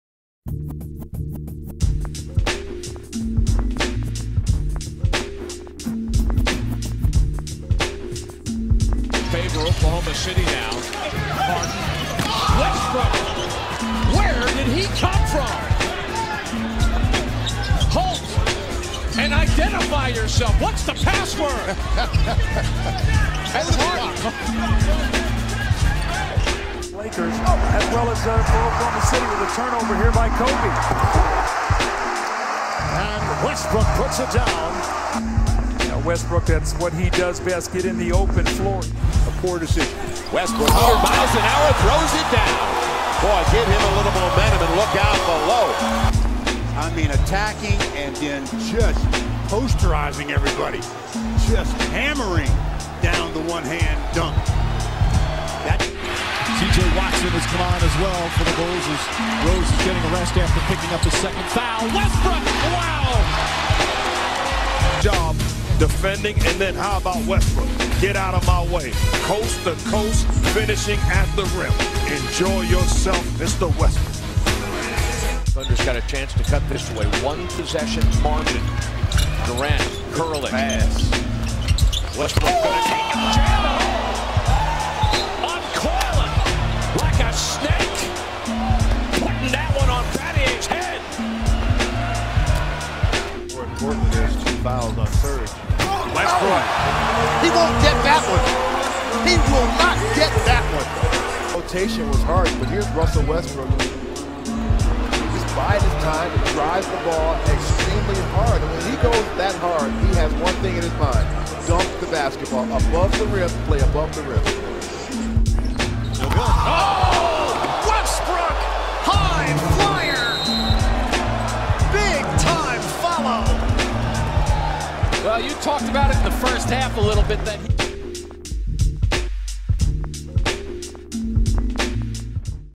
Top 10 Dunkers in the NBA - #10: Russell Westbrook [HD]
Wegobomber Basketball's special look at the top 10 dunkers in the NBA.  At #10 is Russell Westbrook of the Oklahoma City Thunder.  Here is a compilation of some of Westbrook's top dunks over the 2009-10 and 2010-11 seasons.

for insightful NBA analysis, commentary, rankings and Top Dunks of the Week and Year.